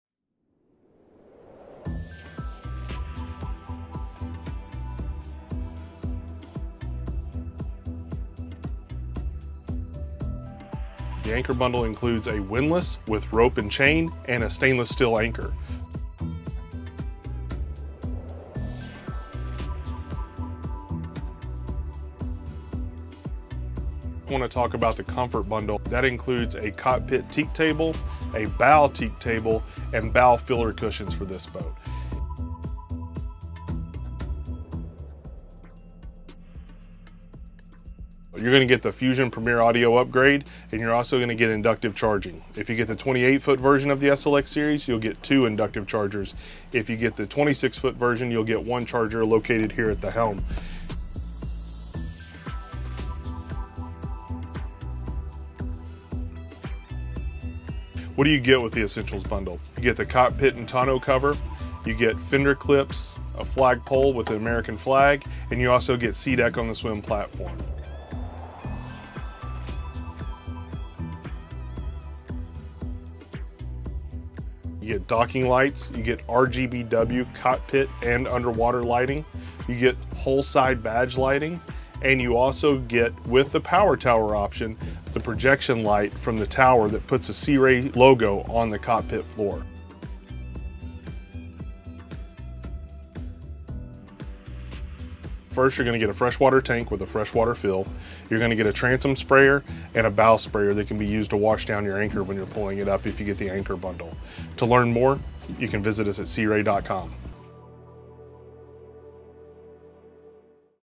SLX Bundle Package Overview | SLX Model Family | Sea Ray Boats
Learn more about our bundling packages offered on the SLX 280 or 260 model families.
Sea Ray is more than buying a boat; it’s embracing a lifestyle. Tune in to learn about Sea Ray’s latest advancements, explore how-to videos and be the first to know about new product announcements.